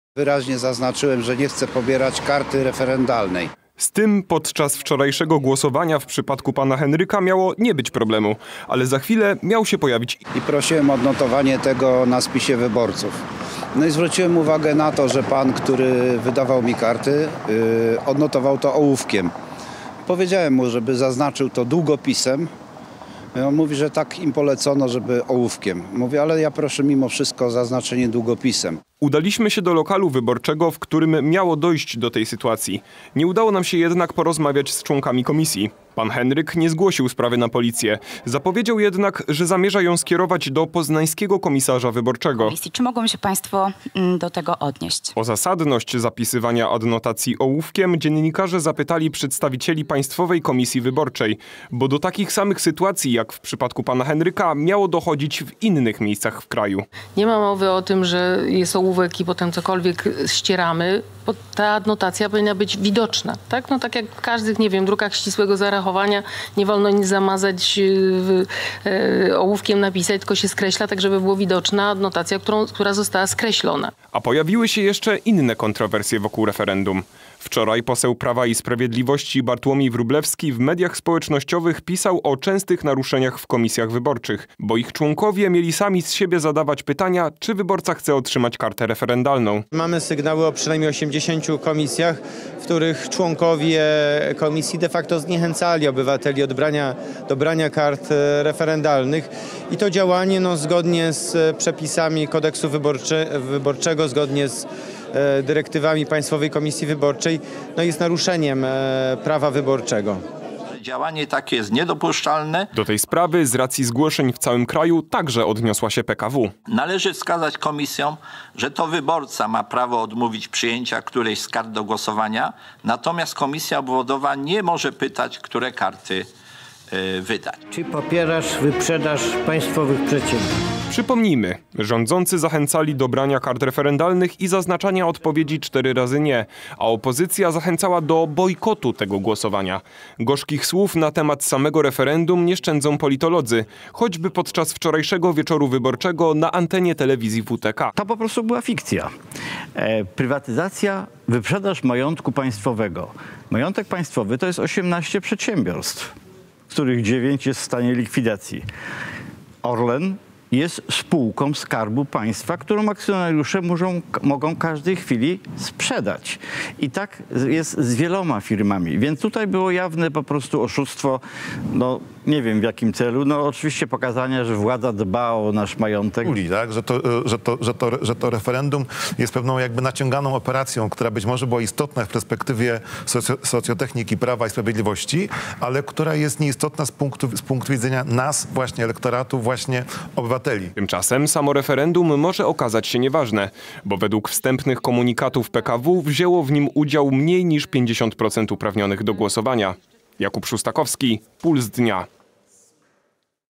Kontrowersje wokół referendum. W tle ołówek
Wybory nie obyły się bez kontrowersji, jeśli chodzi o wydawanie kart do referendum. Ci, którzy nie chcieli ich brać, zgłaszali rozmaite problemy. Zamieszanie, które powstało, jeszcze w czasie głosowania komentowała Państwowa Komisja Wyborcza.